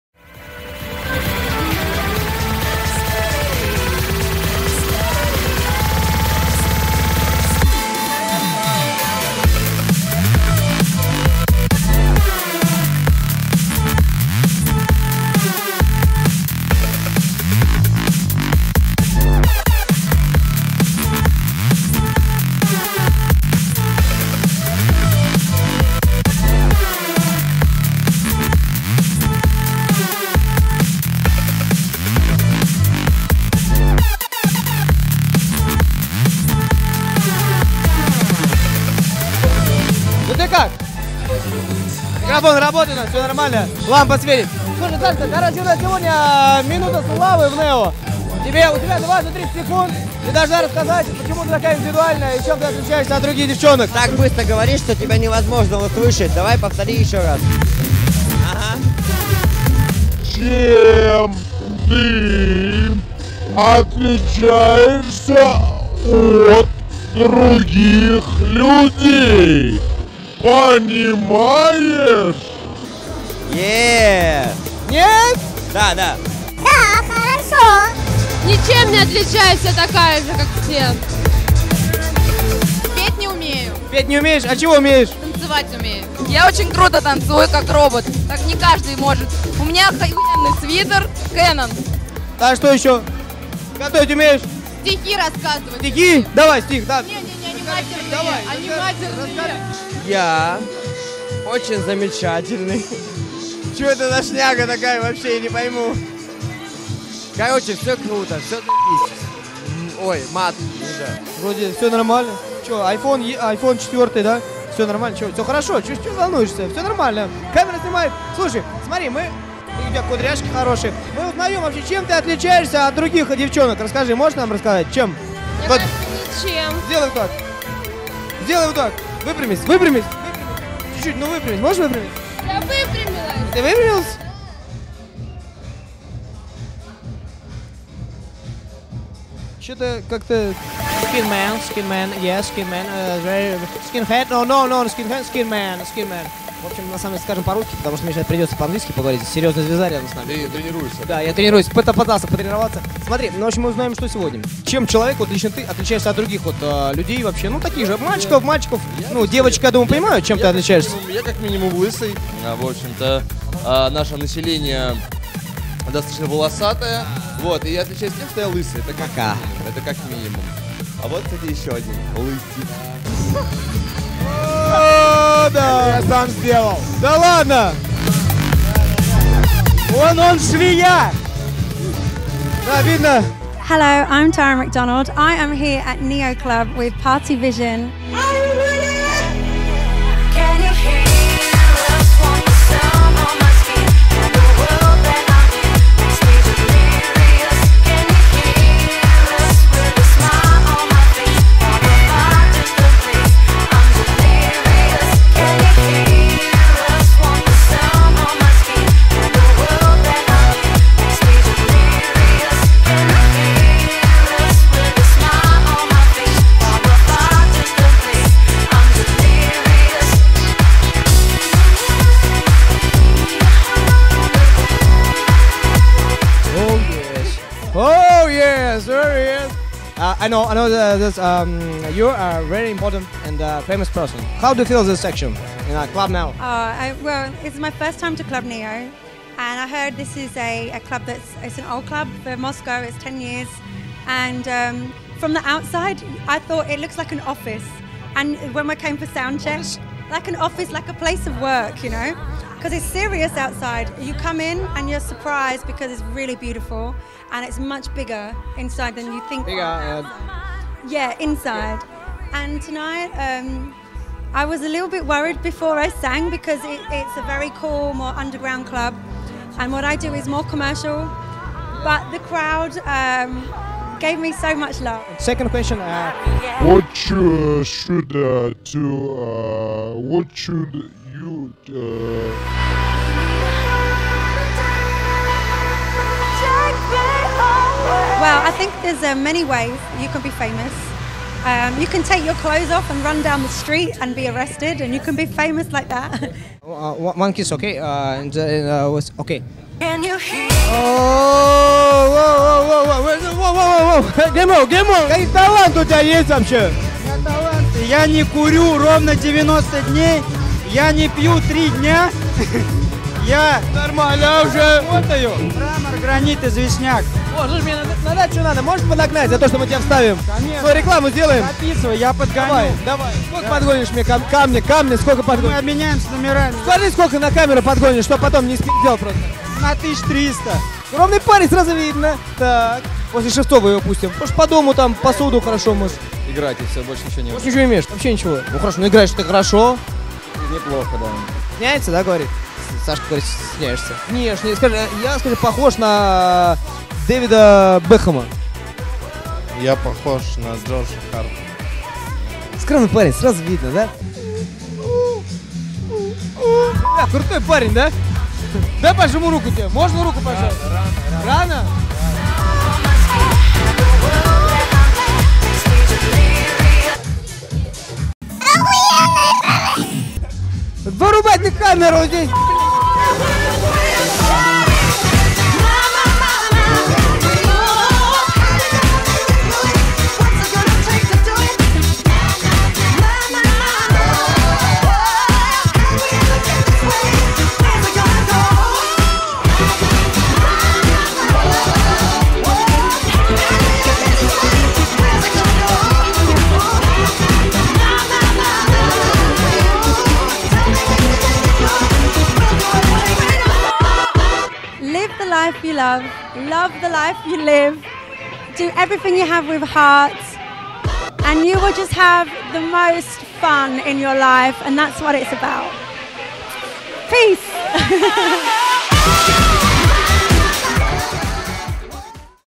Tara McDonald в клубе NEO
Потрясающая и обворожительная Тара МакДональд выступила со своим концертом в клубе НЕО, а также поделилась с гостями своими секретами успешности и популярности. Кстати, Мирону тоже перепало =)